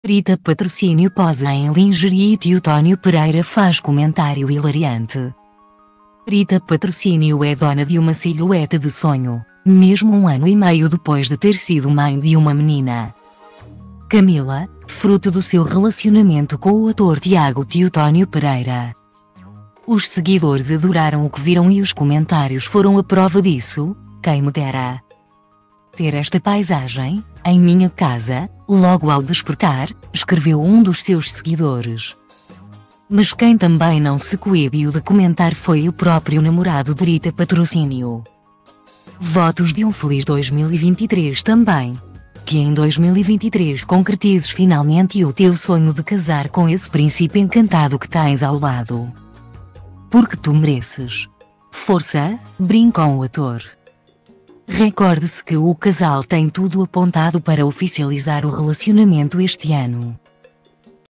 Rita Patrocínio posa em lingerie e Teotónio Pereira faz comentário hilariante
Rita Patrocínio é dona de uma silhueta de sonho, mesmo um ano e meio depois de ter sido mãe de uma menina,Camila, fruto do seu relacionamento com o ator Tiago Teotónio Pereira. Os seguidores adoraram o que viram e os comentários foram a prova disso: “Quem me dera,